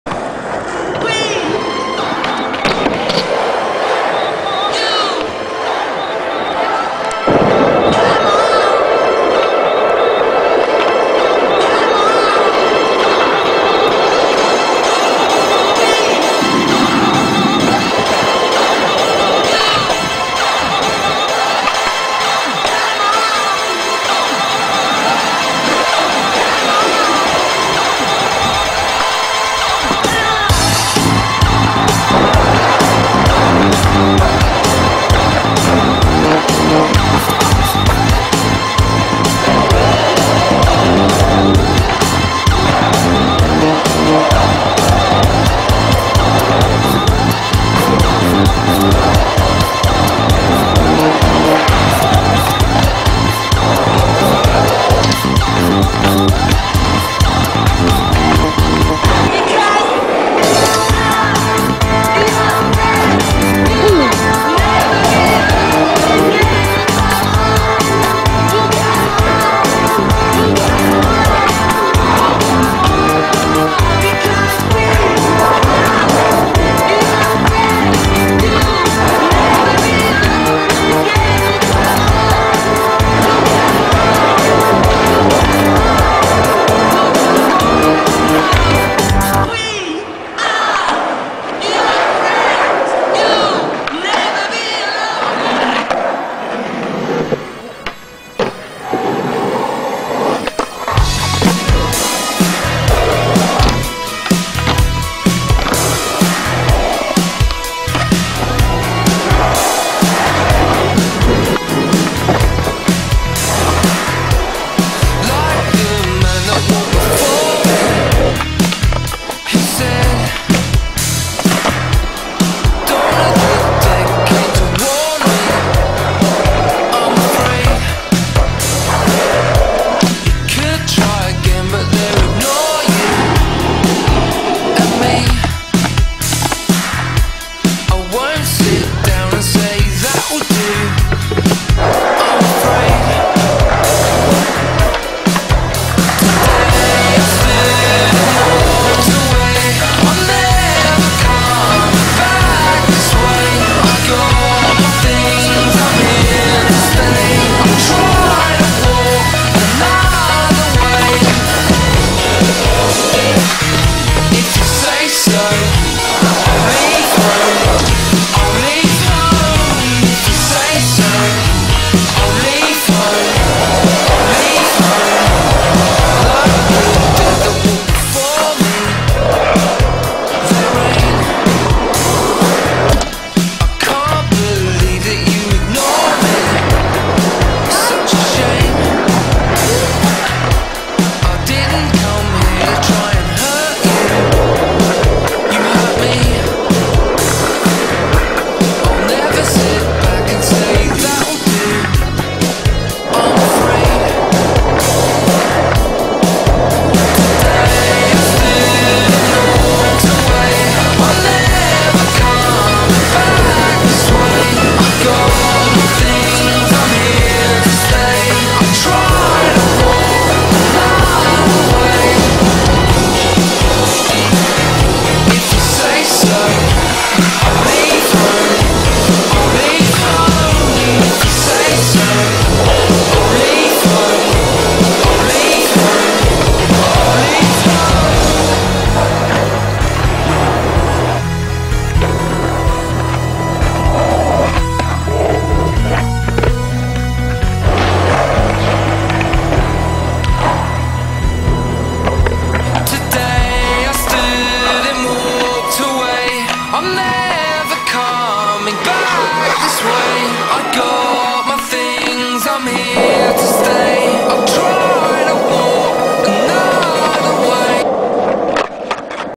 skateparktour august09
skateparks: nuremberg, erlangen, ansbach

skating by: Matthias "mat" Brenner
                    Dominik "nic" Brenner